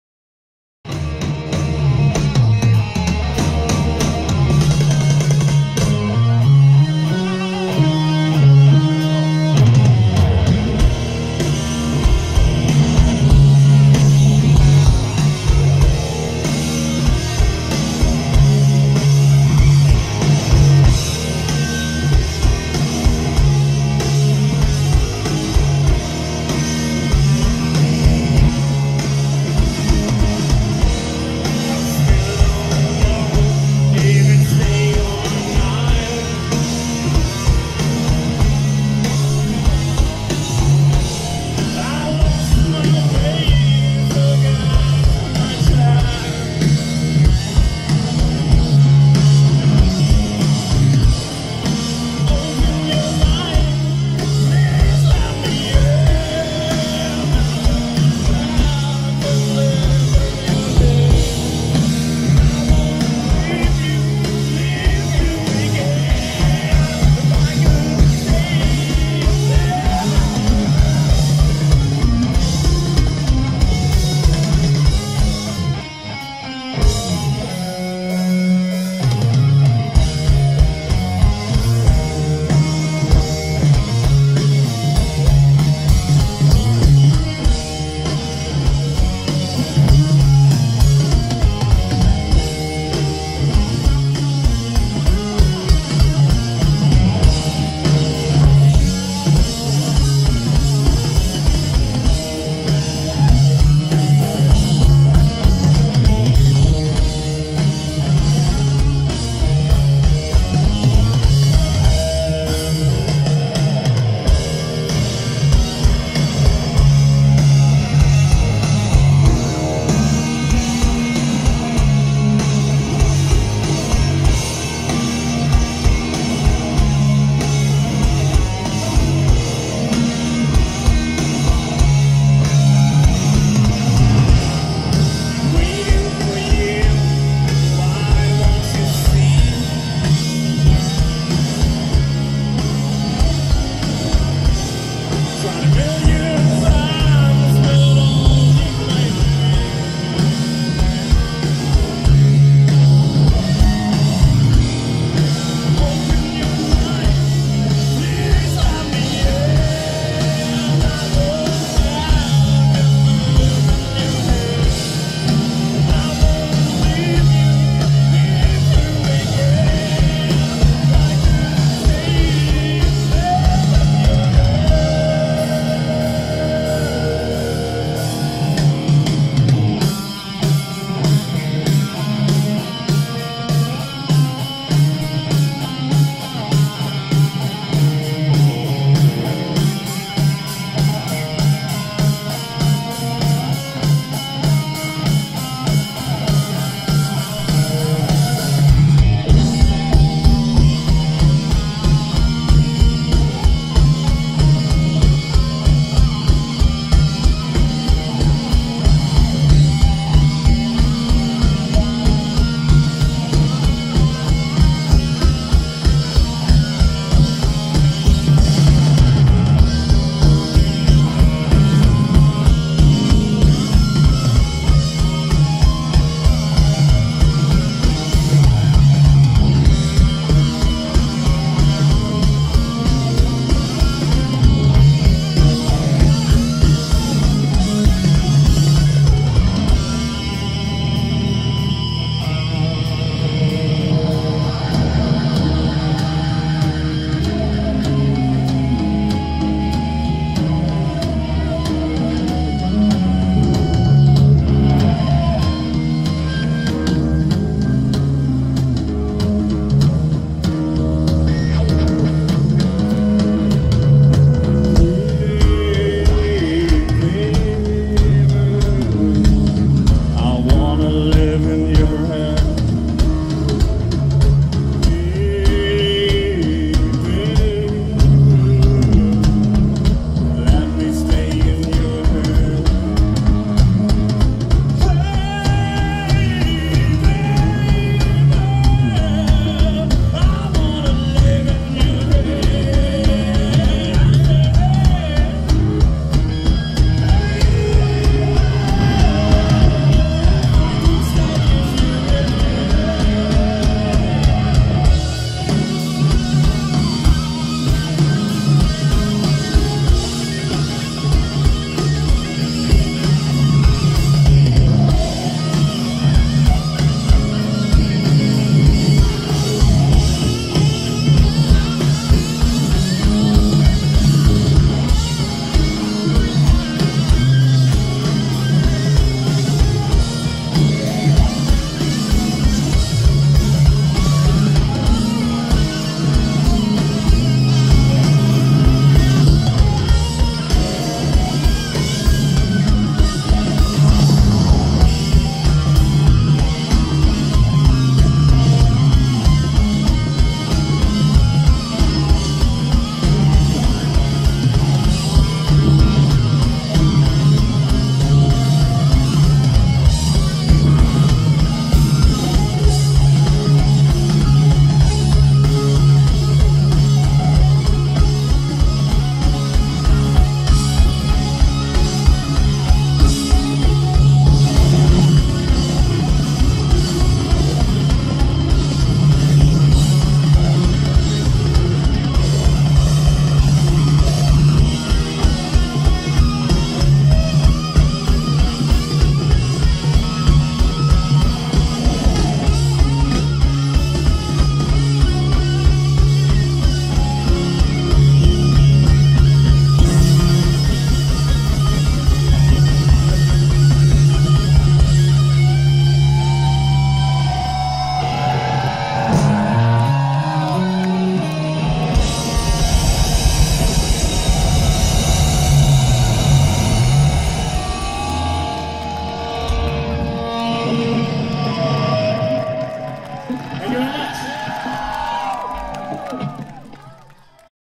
Devilstone Open Air 2013 - Kadavar (live)
Масштабный четырехдневный фестиваль Devilstone Open Air 2013, собирающий на берегу реки Швентойи любителей творчества, rock- и metal-музыки, а также активного отдыха, отгремел в литовском городке Аникщяй 11-14 июля. Фоторепортаж на